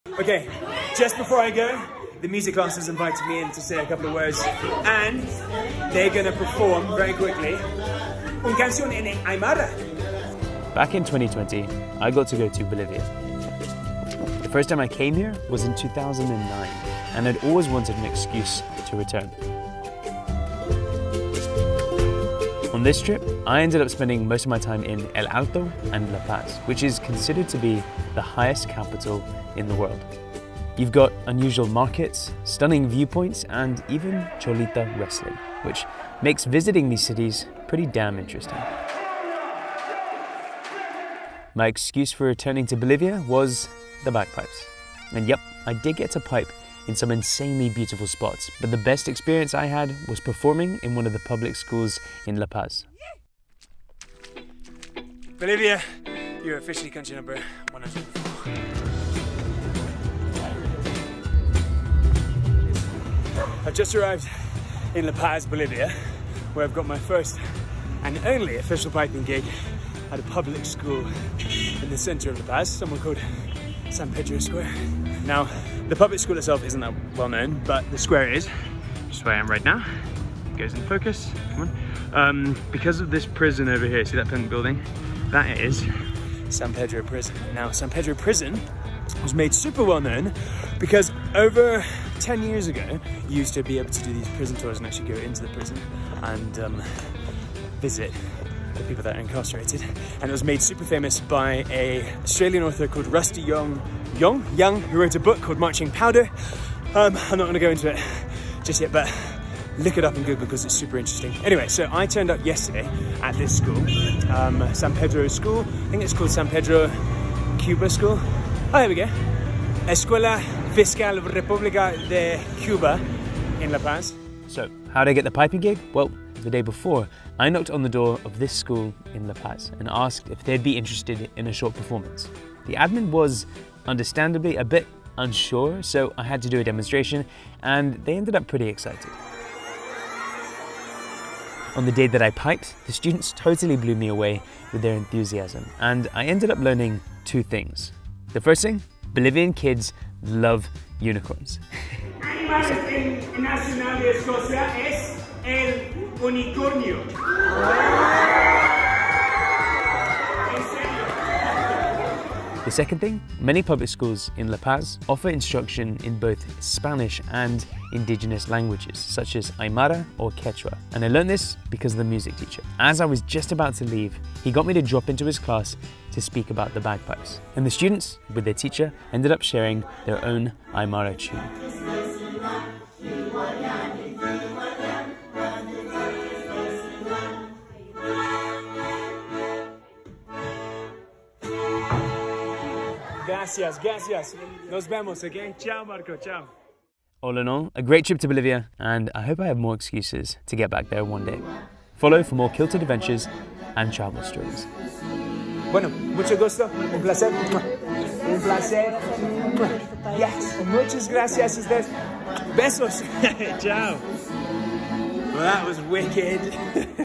Best thing to do in La Paz?! | Bagpiping in a School!
Come along to the city of La Paz (Bolivia) and find out how my second visit to Bolivia landed me on a random trip to a local school.